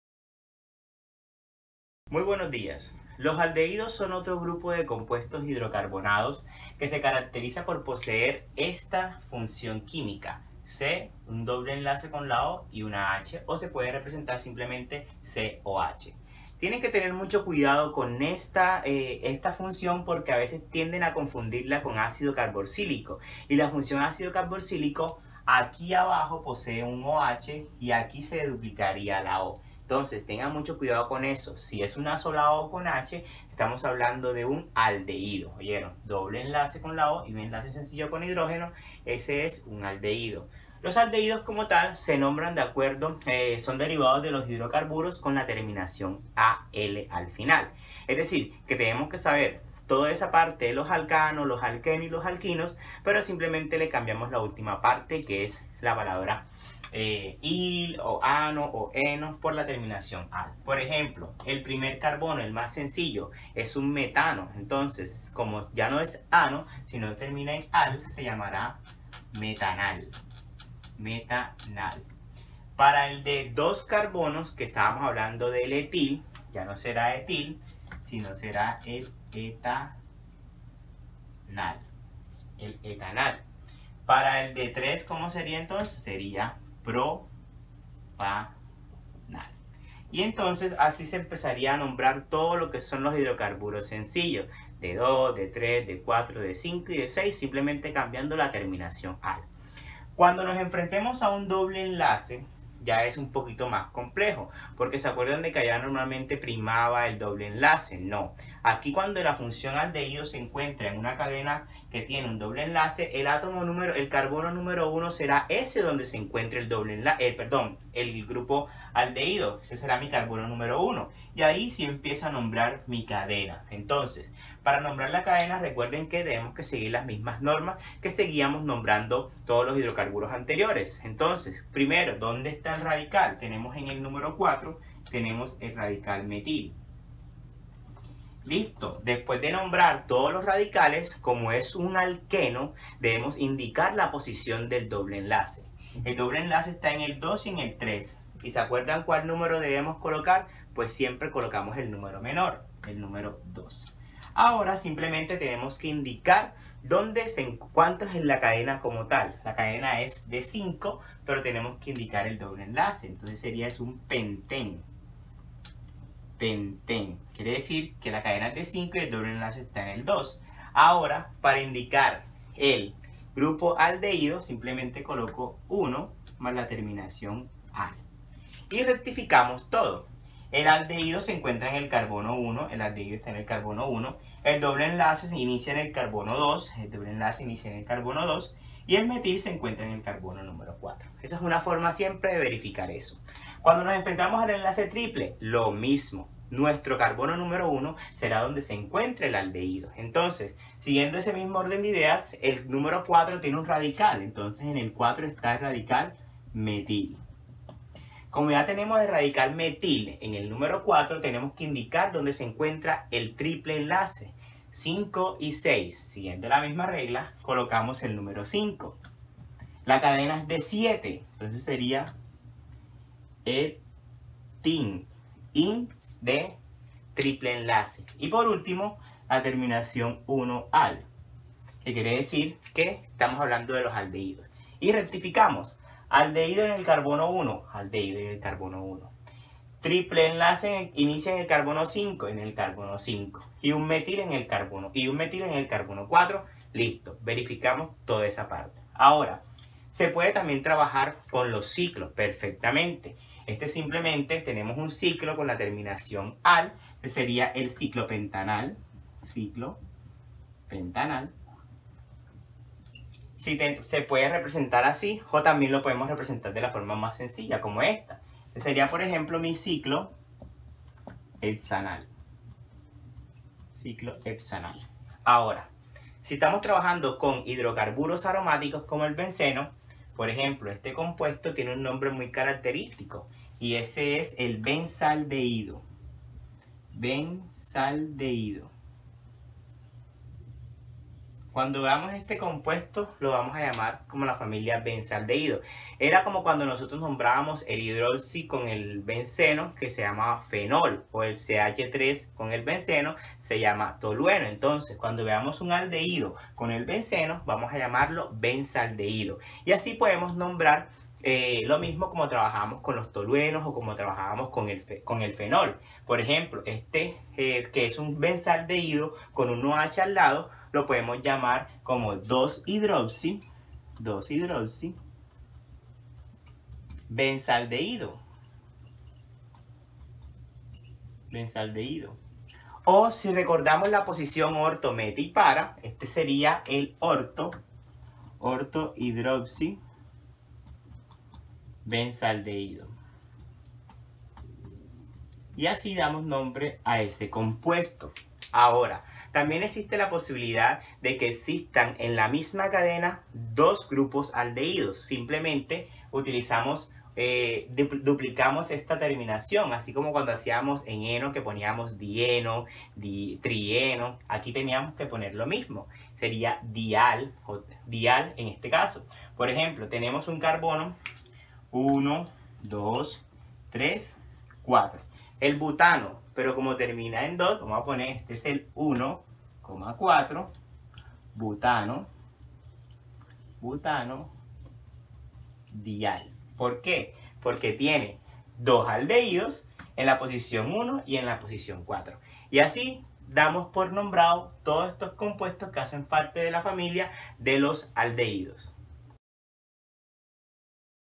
FUNCIÓN QUÍMICA ALDEHÍDO NOMENCLATURA IUPAC- Química Profe Germán
Los aldehídos son compuestos orgánicos caracterizados por poseer el grupo funcional -CHO (carbonilo).​ Un grupo carbonilo es el que se obtiene separando un átomo de hidrógeno del formaldehído. Como tal no tiene existencia libre, aunque puede considerarse que todos los aldehídos poseen un grupo terminal carbonilo.
Aprendamos juntos a darle nombre a estos compuestos orgánicos.